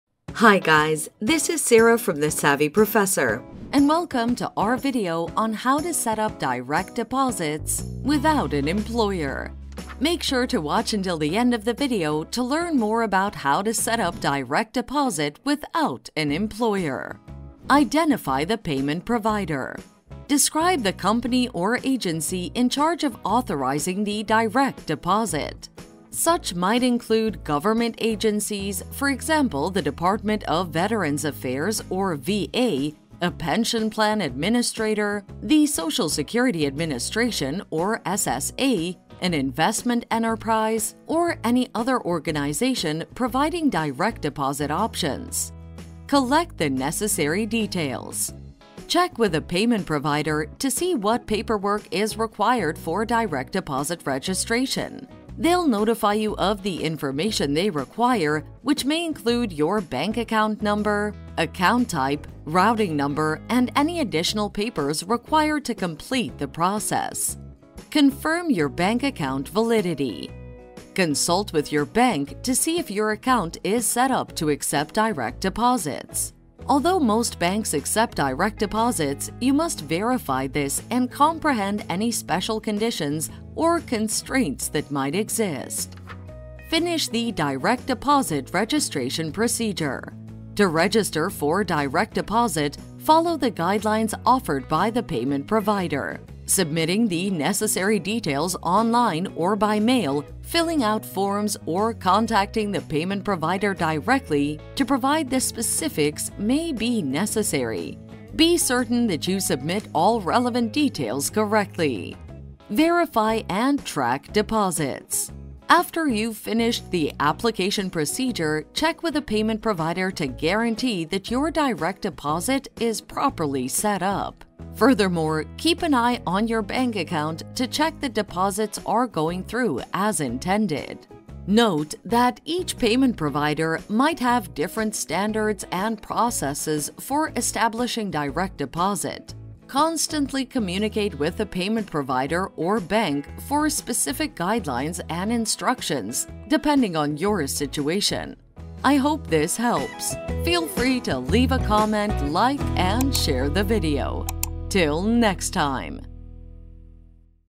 How to Set Up Direct Deposit Without Employer (A Step-by-Step Guide)
How to Set Up Direct Deposit Without Employer (A Step-by-Step Guide). Welcome to our video on How to set up direct deposit without an employer.

How to set up direct deposit without employer
Identify the payment provider
Describe the company or agency in charge of authorizing the direct deposit.

Collect the Necessary Details
Check with the payment provider to see what paperwork is required for direct deposit registration.

Confirm Your Bank Account Validity
Consult with your bank to see if your account is set up to accept direct deposits.

Finish the Direct Deposit Registration Procedure.
To register for direct deposit, follow the guidelines offered by the payment provider.

Verify and track deposits.
After you've finished the application procedure, check with the payment provider to guarantee that your direct deposit is properly set up.